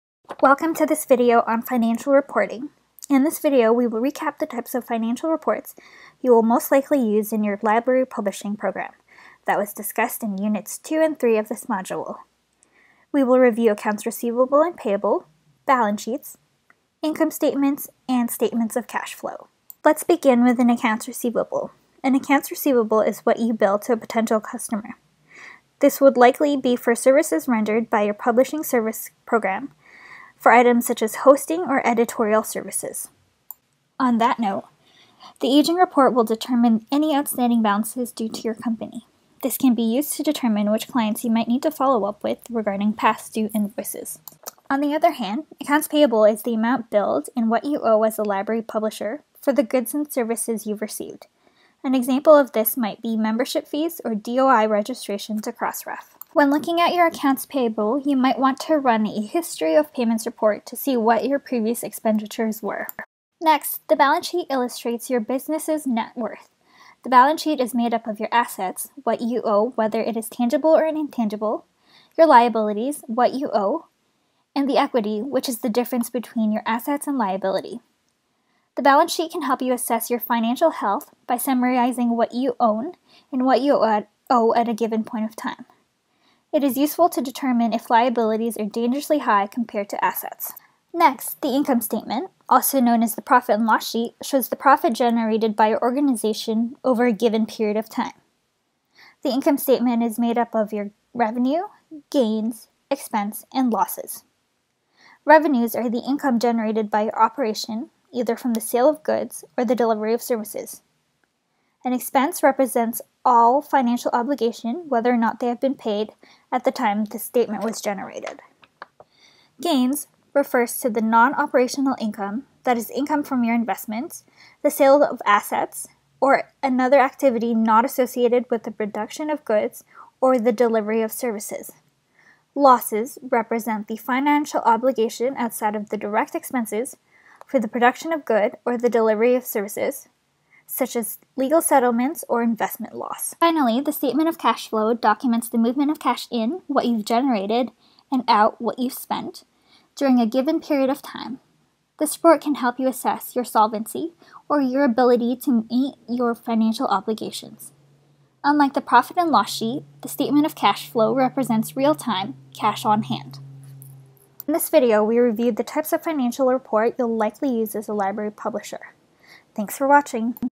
Sustainability of your Library Publishing Program: Module 2 - Financials
In this video we will recap the types of financial reports you will most likely use in your library publishing program, that were discussed in units 2 and 3 of this module. We will review accounts receivable and payable, balance sheets, income statements, and statements of cash flow.